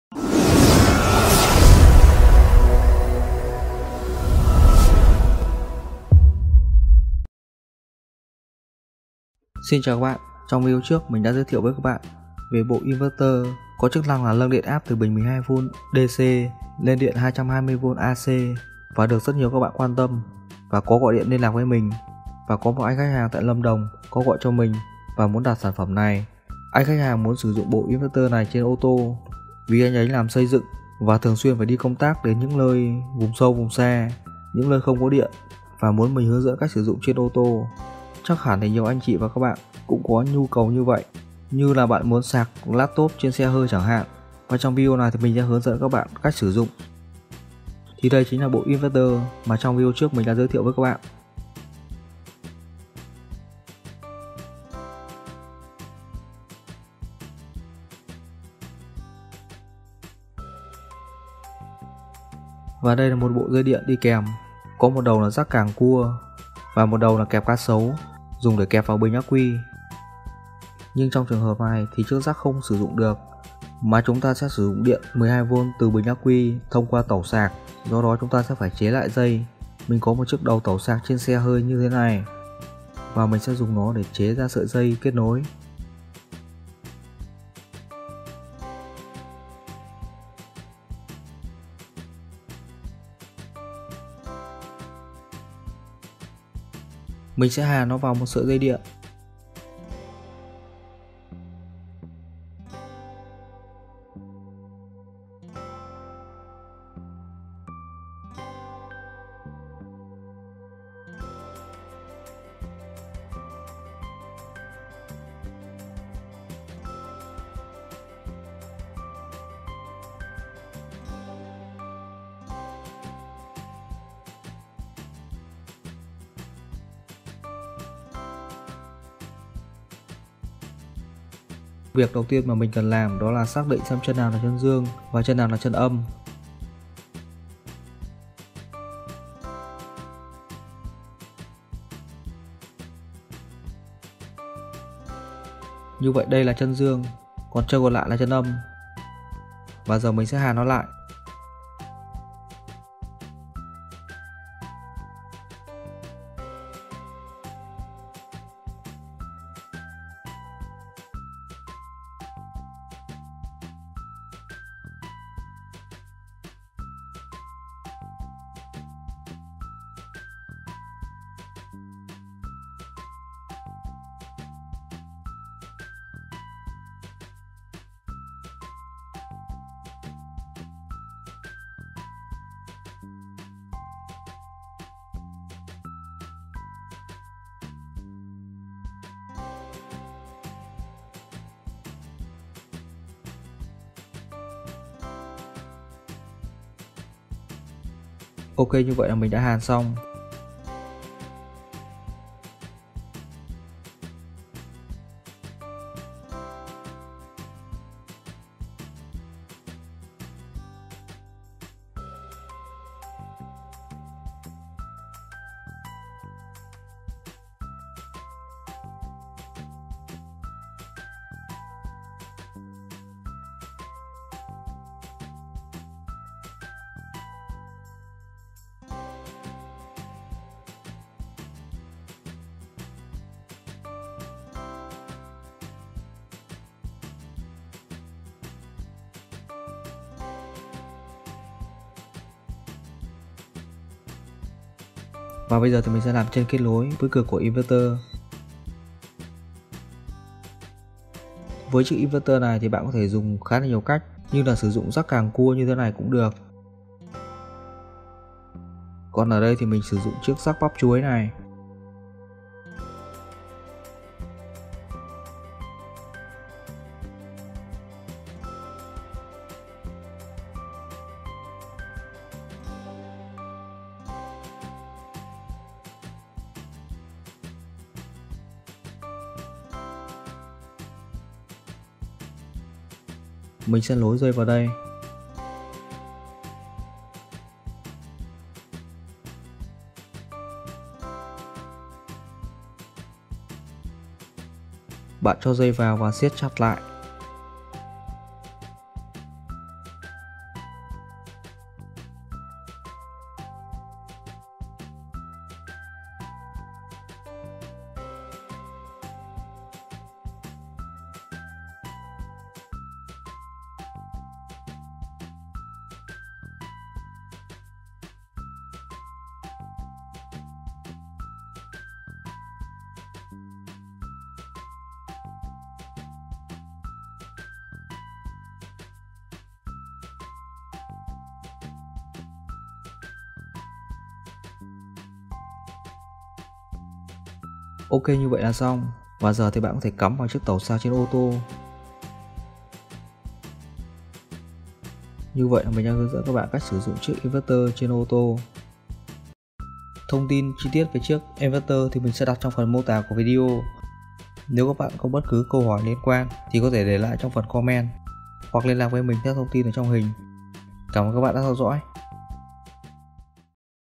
Sạc Laptop trên xe hơi - Xe Hải Đăng
Sạc Laptop trên xe hơi

Video mình hướng dẫn các bạn chế một chiếc sạc Laptop dùng trên xe hơi.

*** Cảm ơn các bạn đã luôn ủng hộ và đón xem video của Xe Hải Đăng

© Bản quyền thuộc về Xe Hải Đăng
© Copyright by Xe Hải Đăng ☞ Do not Reup